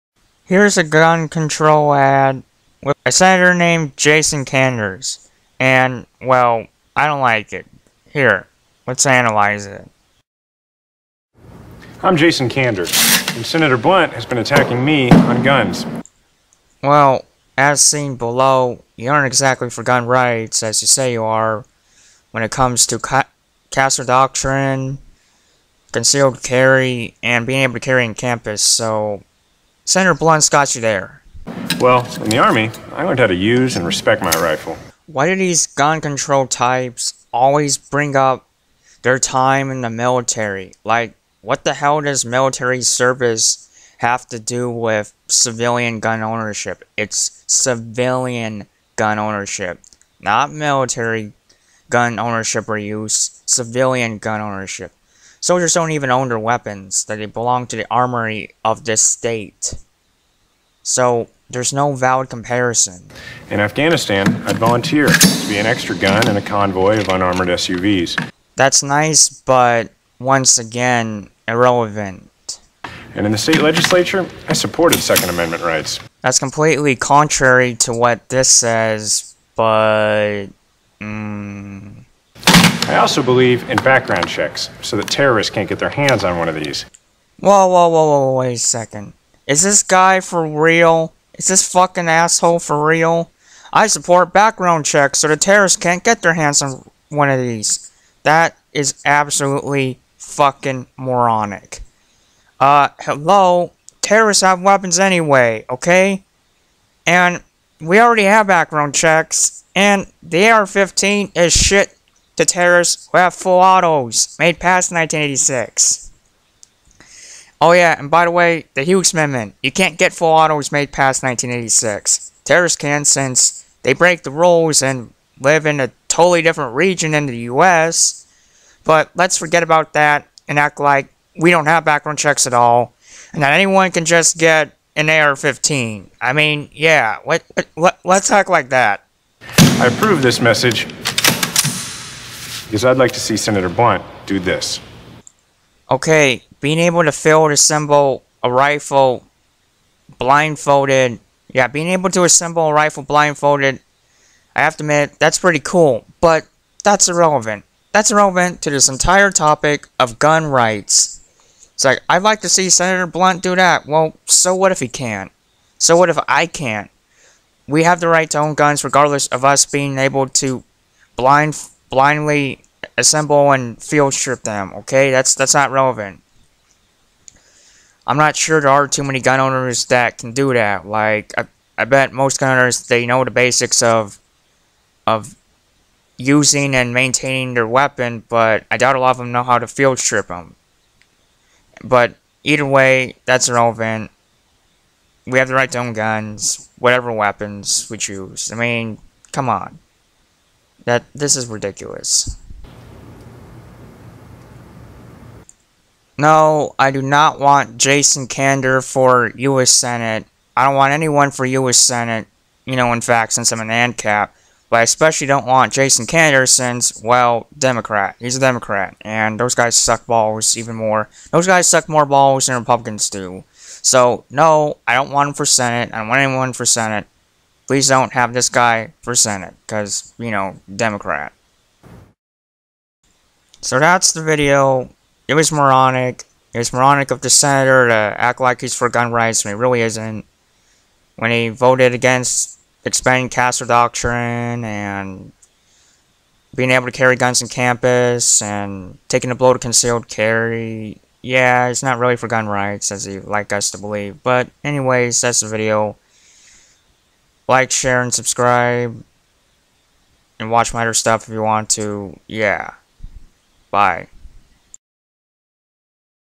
Jason Kanders Doesn't Know Guns
Yes, I know this is a 2 year old political ad but since the war against guns rages on I still find it relevant.

I'm on Rumble now!

Like what I make? Free free to pledge!

Bitcoin Address: 1JVkpnMnfdXQ1ekMGtfaMPL91kDpJ5V2aK

I'm on Bitchute now!